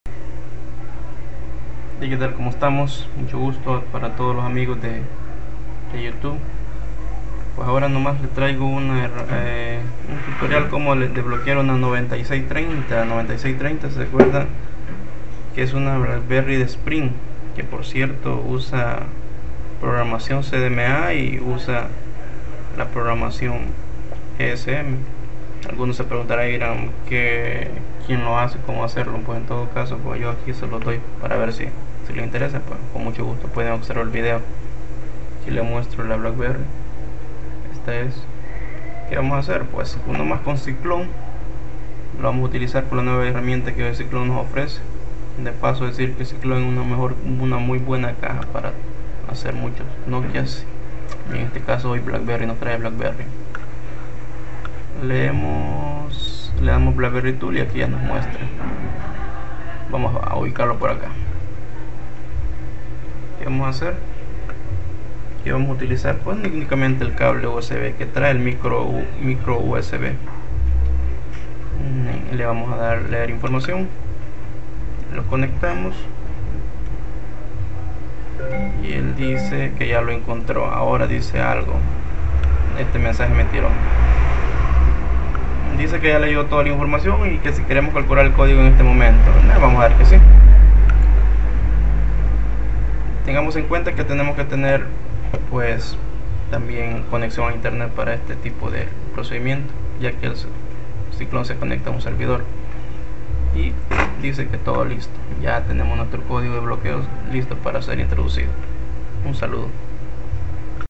Liberar Blackberry by Cyclone Box
Tutorial para Calcular el Codigo de Liberacion de Blackberry.
Tomar encuenta que debemos tener Internet. Esto no es un software libre ya que trabaja con su Box. Segun puedes ver en el vieo.